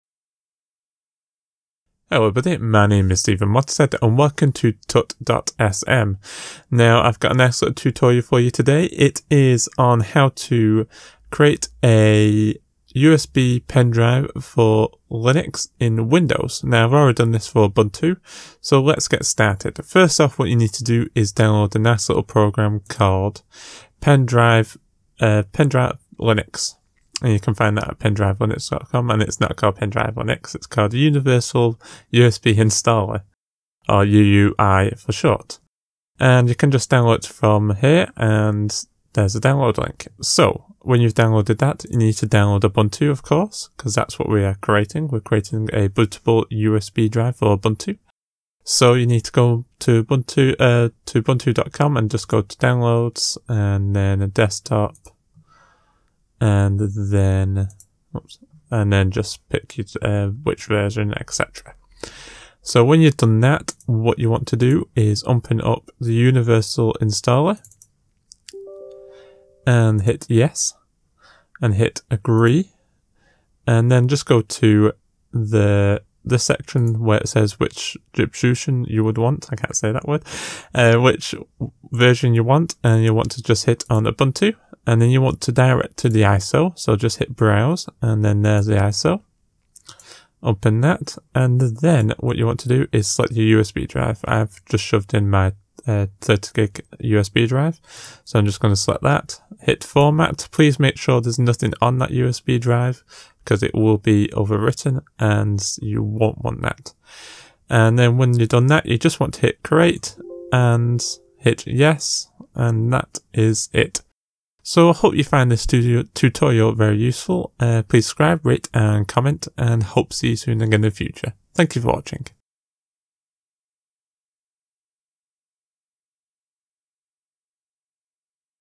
Ubuntu - Create bootable USB (Windows)
-- Ubuntu - Create bootable USB (Ubuntu) --

In this quick tutorial I am going to show you how to create a bootable usb stick for Ubunut in Windows OS.

If you find this helpful and you would like to support me. Please send me a something vie